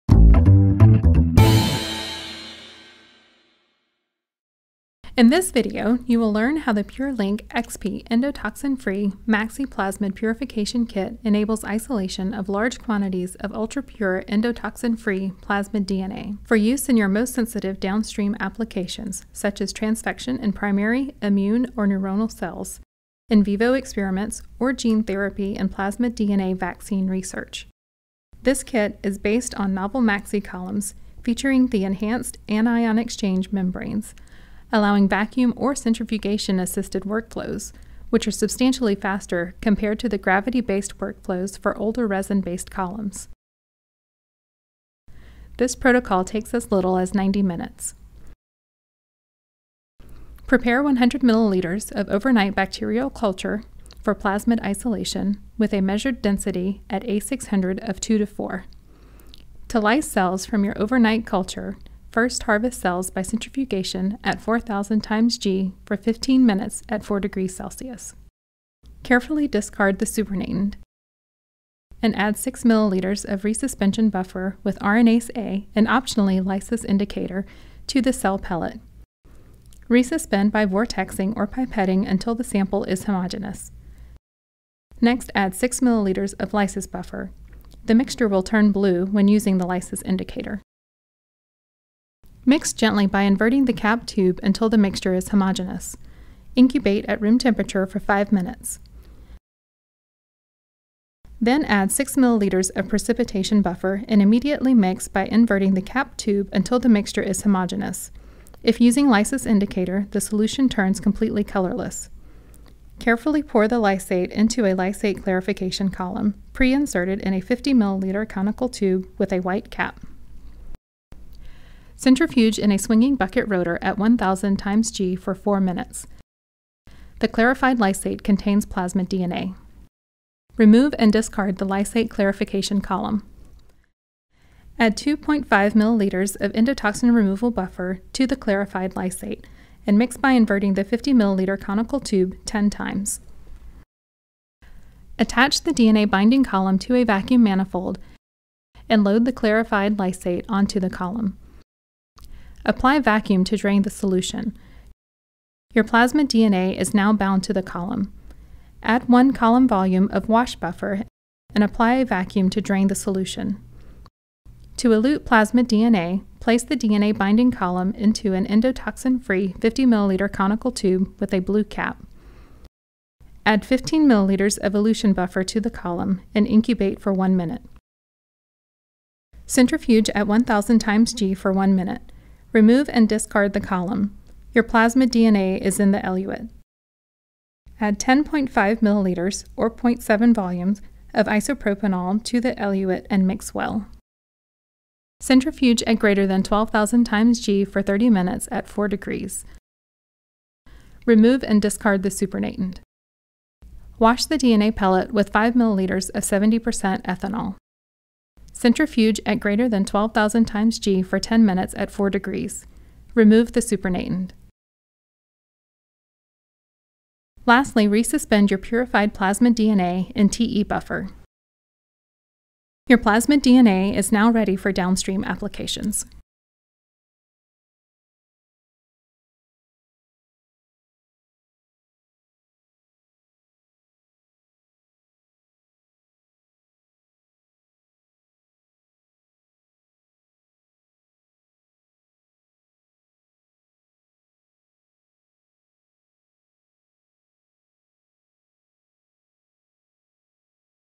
Endotoxin-Free Plasmid Prep | PureLink XP Maxi Kit Tutorial
Learn how to perform endotoxin-free plasmid prep using the PureLink XP Endofree Maxi Plasmid Purification Kit from Thermo Fisher Scientific. This video tutorial demonstrates the
full workflow for isolating large quantities of ultrapure plasmid DNA that is free from endotoxins, making it ideal for sensitive applications such as transfection in primary immune cells, neuronal cells, in vivo studies, gene therapy, and plasmid DNA vaccine research. The protocol uses enhanced anion exchange membranes with vacuum or centrifugation-assisted workflows to save time compared to traditional gravity-based methods.

Chapters:
00:00 Introduction to Endotoxin-Free Plasmid Prep
00:30 Preparing Bacterial Cultures
01:10 Cell Lysis and Clarification
01:50 Binding DNA to Maxi Columns
02:30 Washing and Endotoxin Removal
03:10 Elution and DNA Recovery
03:50 Downstream Applications
Thermo Fisher Scientific provides trusted plasmid DNA purification solutions for research and therapeutic applications.